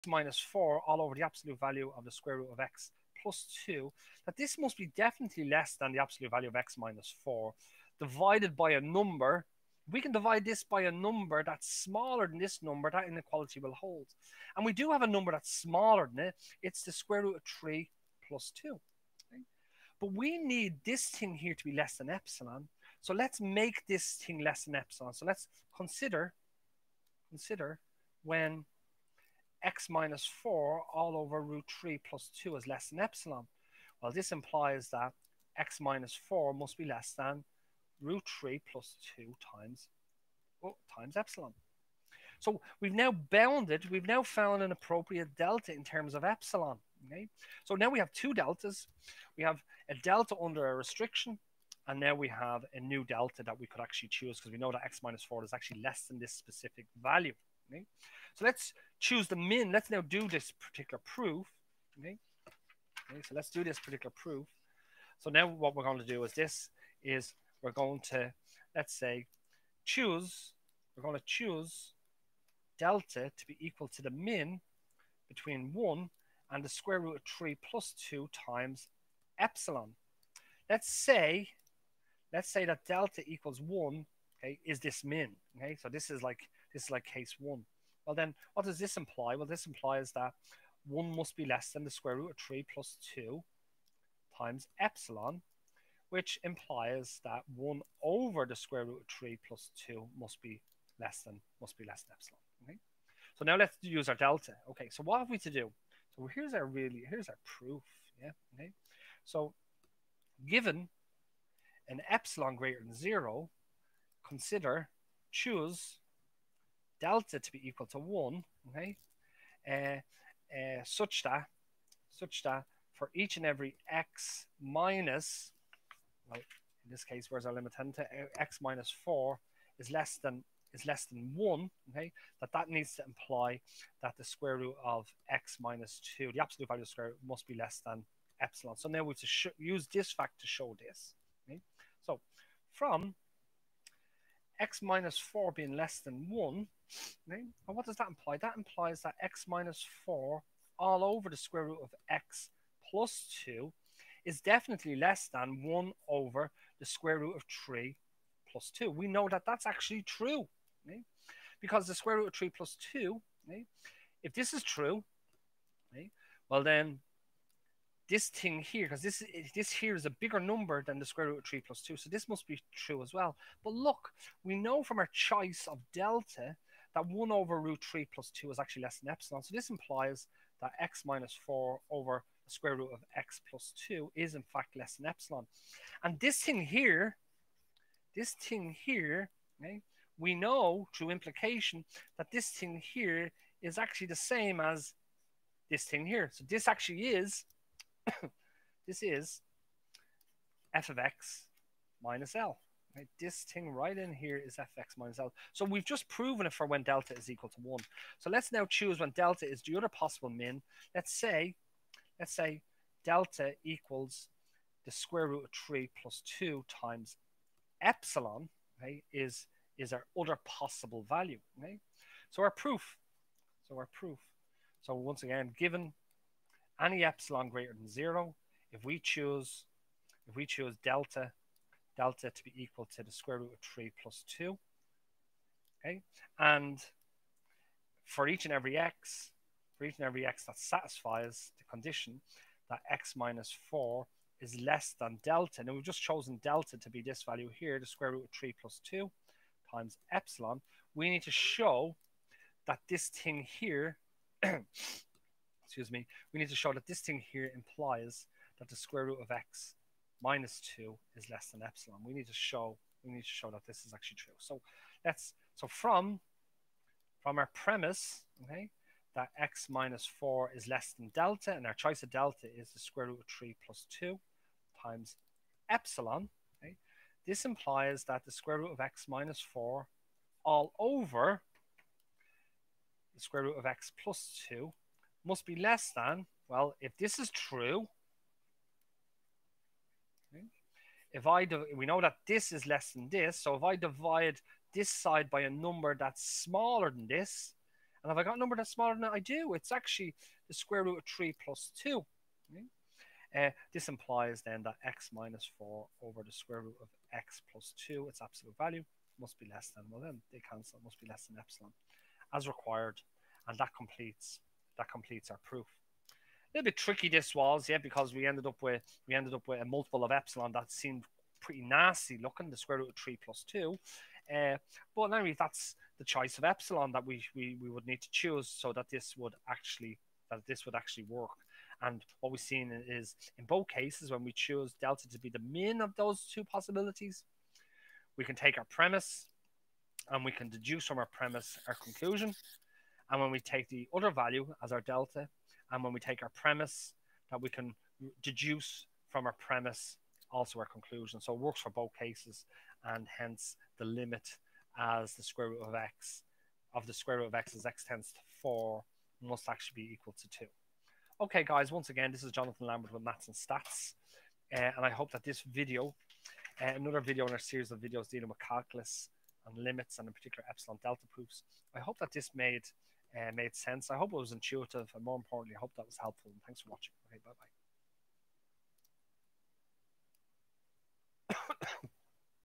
Calculus - Limits (Epsilon Delta Proofs): Limit of a Square Root Function (Example 1) - Part 2
In this video, we present an Epsilon Delta proof for the Limit of a Square Root function.